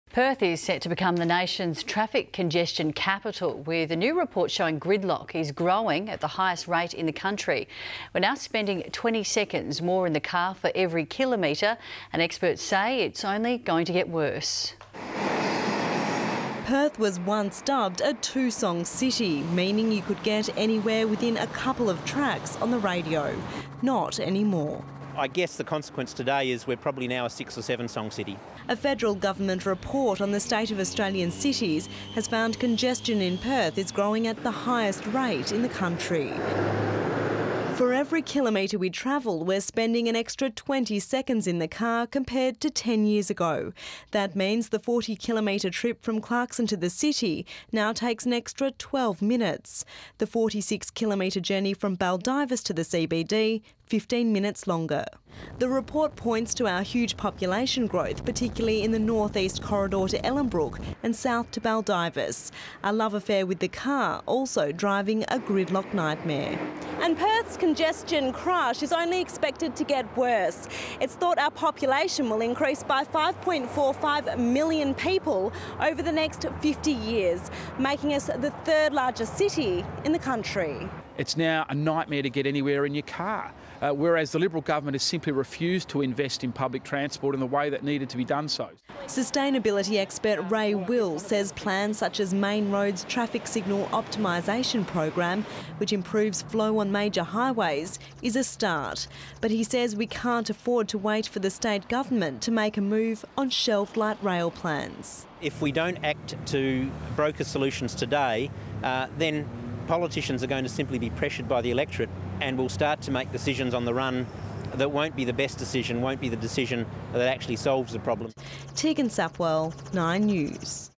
Gridlock Woes | 9 News Perth
Perth is set to become the nation's traffic congestion capital with a new report showing gridlocks are growing at the highest rate in the country. We're now spending 20 seconds more in the car for every kilometre and experts say it's only going to get worse.